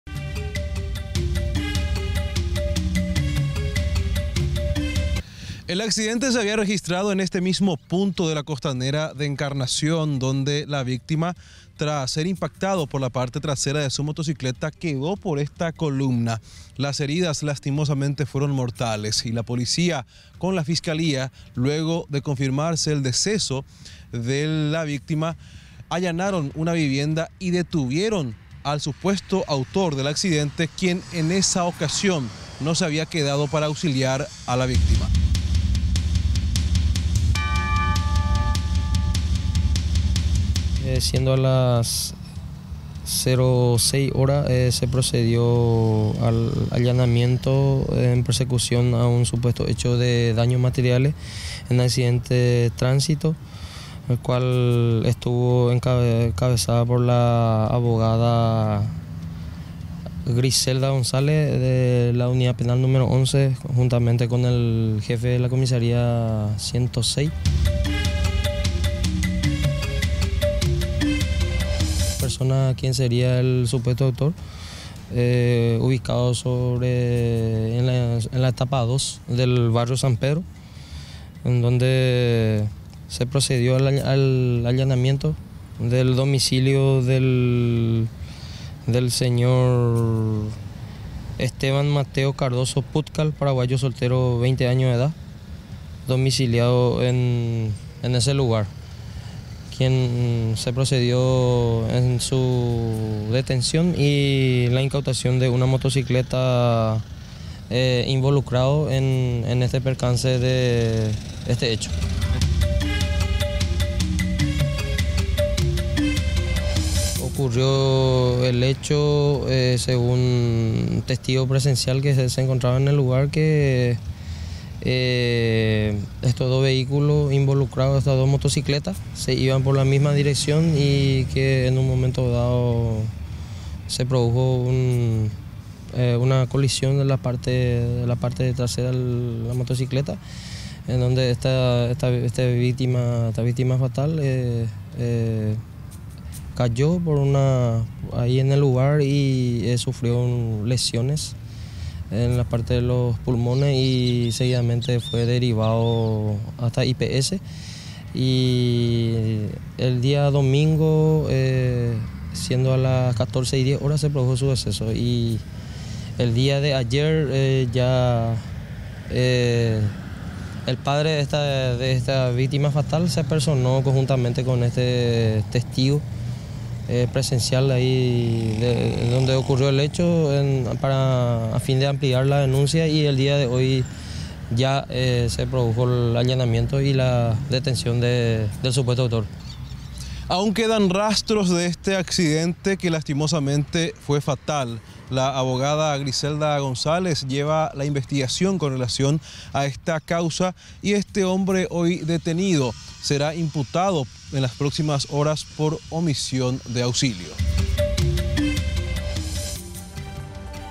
DETENIDO TRAS ACCIDENTE FATAL
EL HOMBRE HUYÓ TRAS CHOQUE FATAL